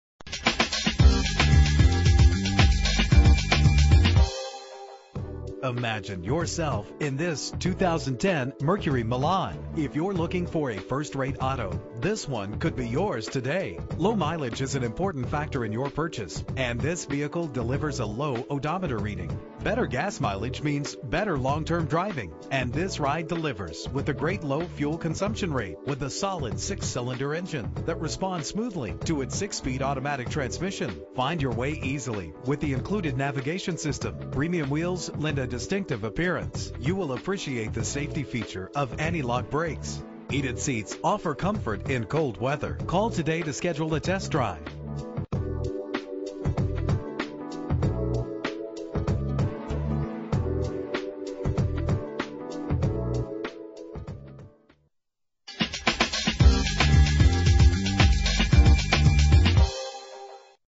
2010 Mercury Milan - Cicero NY
Year: 2010
Make: Mercury
Model: Milan
Engine: 3.0 liter 6 cylinder 24 valve
Transmission: 6-Speed Automatic
Exterior: White
Miles: 9326
Driver's Village